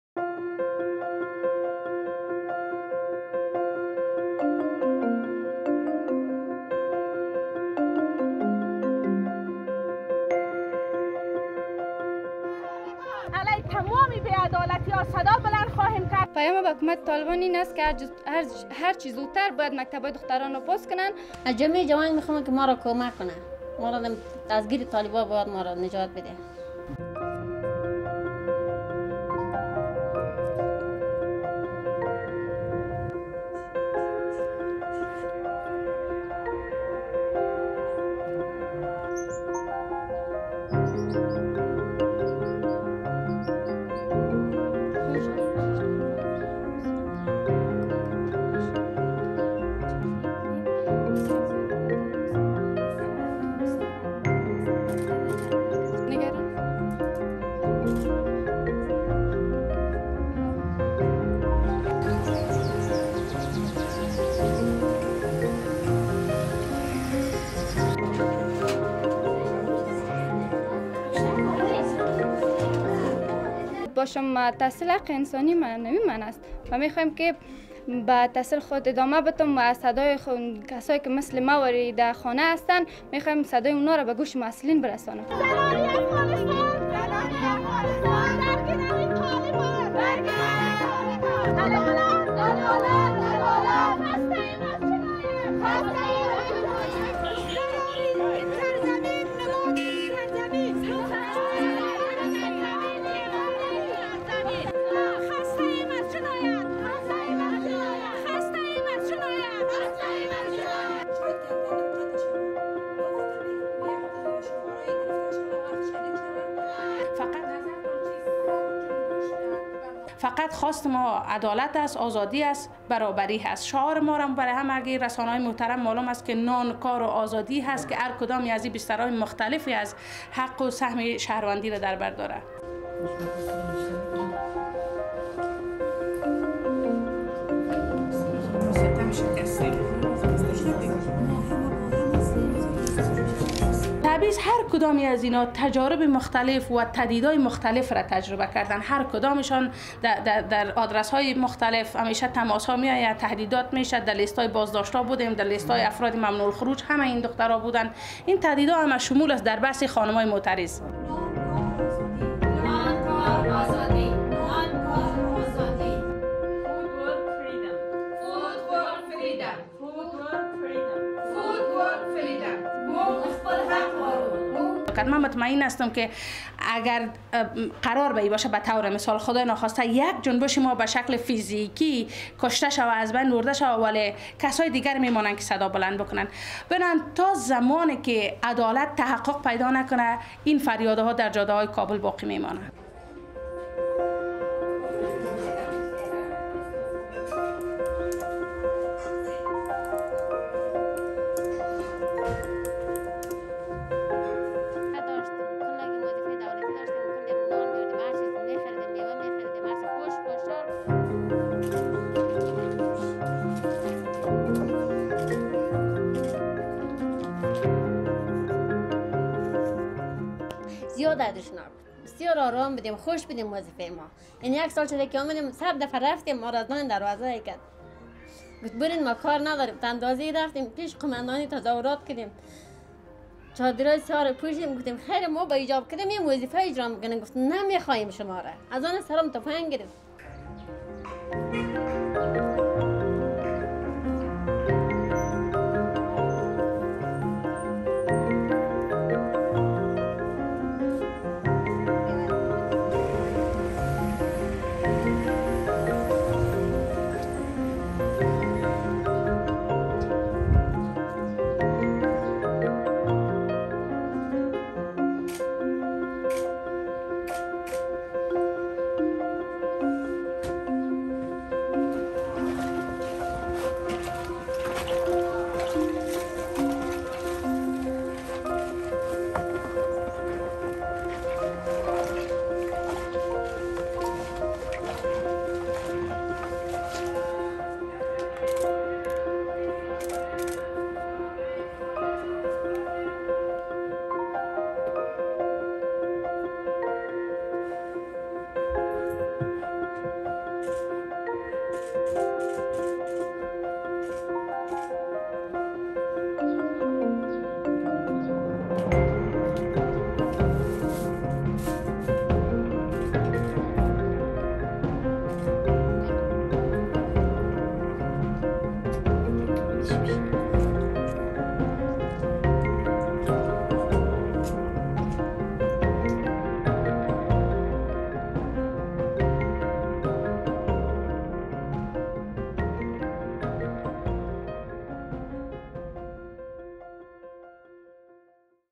Afghan women speak out one year after the Taliban seized power
A year since the Taliban seized control of Afghanistan in August 2021, some Afghan women continue to fight for their basic rights to education and employment. A student, an activist and a former policewoman shared their experiences of adversity since the Taliban kept girls from obtaining secondary education and thousands of women from their jobs.